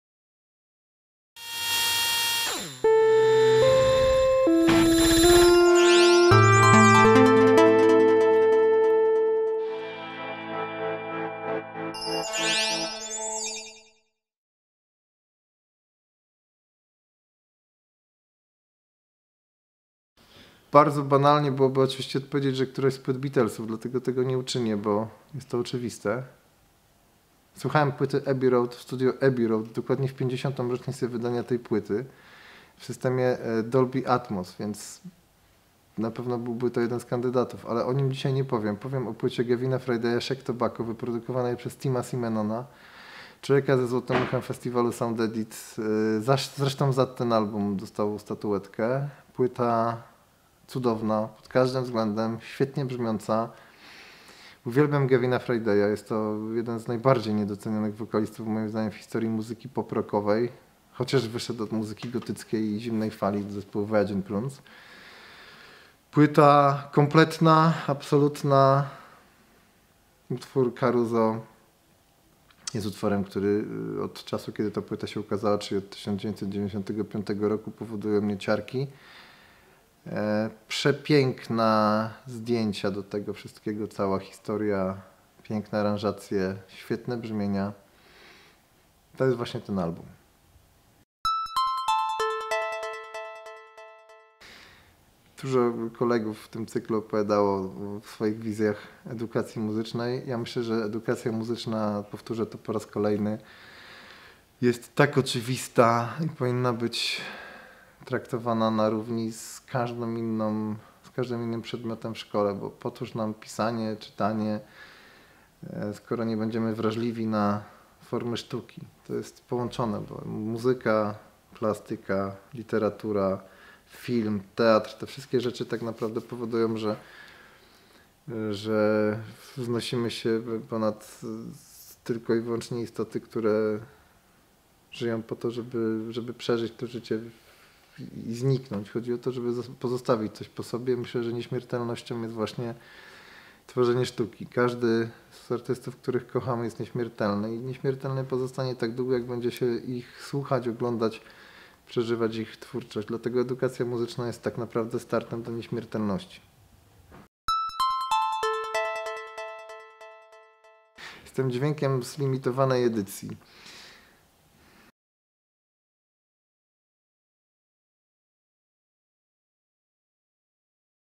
"Jakim Jesteś Dźwiękiem?" - Maciej Werk
"Jakim Jesteś Dźwiękiem?" to seria krótkich filmów i pytań, jakie zadajemy Artystom, nie tylko tym związanym z muzyką.
Produkcja: Fundacja Art Industry 2020